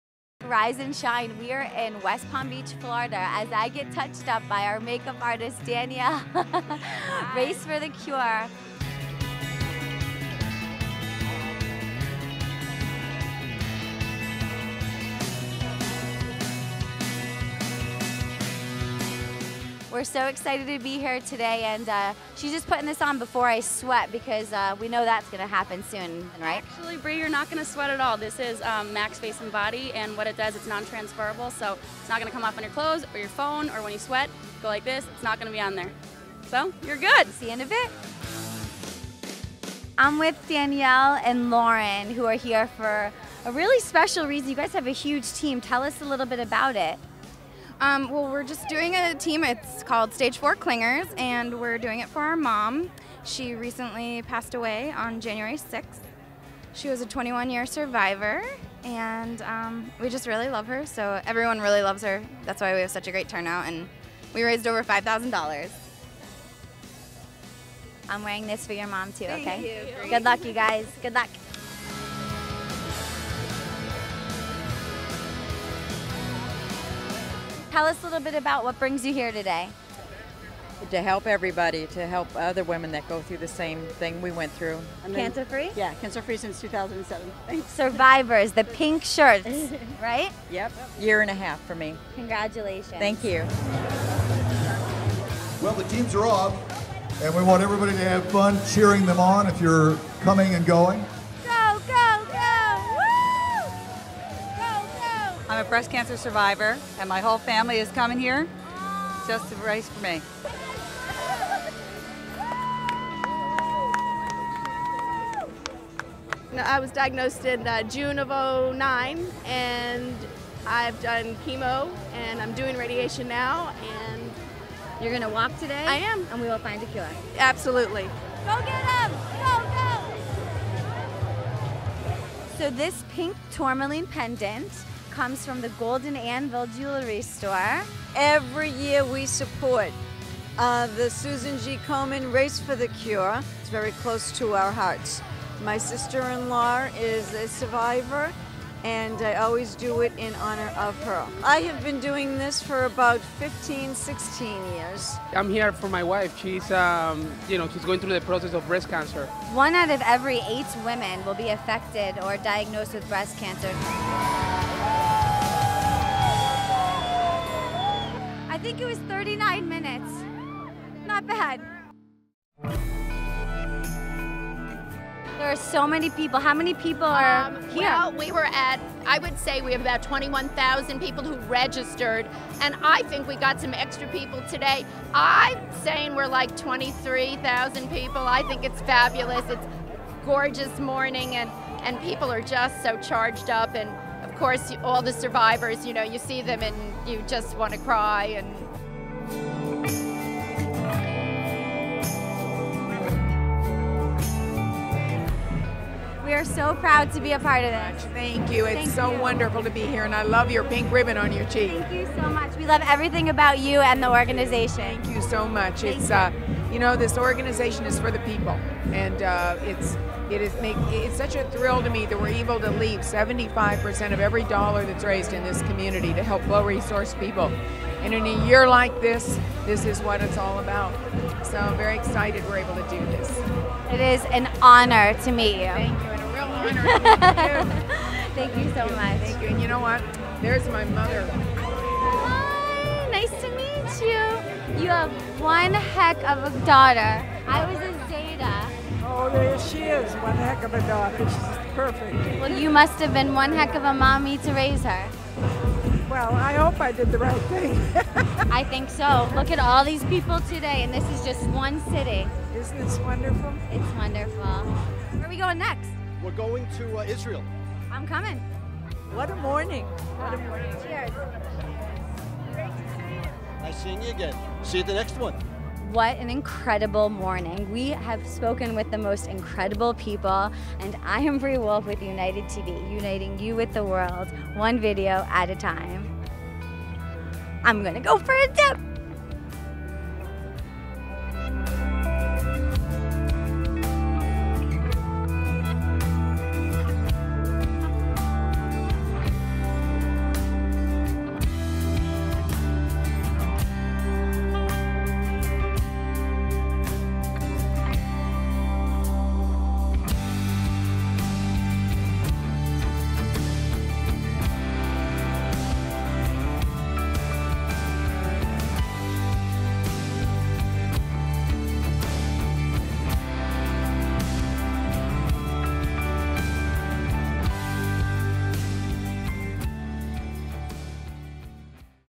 The Susan G. Komen South Florida Race for the Cure® is YOUnited
We are honored to interview founder, Nancy G. Brinker and her mother!! "A world without breast cancer"